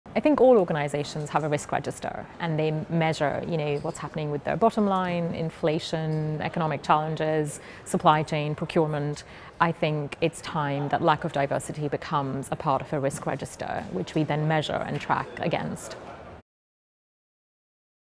Employer webinar | Elevate Equity | Jhumar Johnson's summary
Our Elevate Equity event brought together a number of leading experts in diversity and inclusion to The Open University’s campus in Milton Keynes. In front of a live and virtual hybrid audience, three panels talked about the role of equity in the workplace.

The sessions looked at the recruitment and attraction process, the role of learning and development, and how equitable processes can power better business outcomes.

The three sessions were hosted by diversity thought leader and broadcaster, June Sarpong OBE, who invited the experts to share their viewpoints on delivering equity at work and questions from the audience were answered throughout.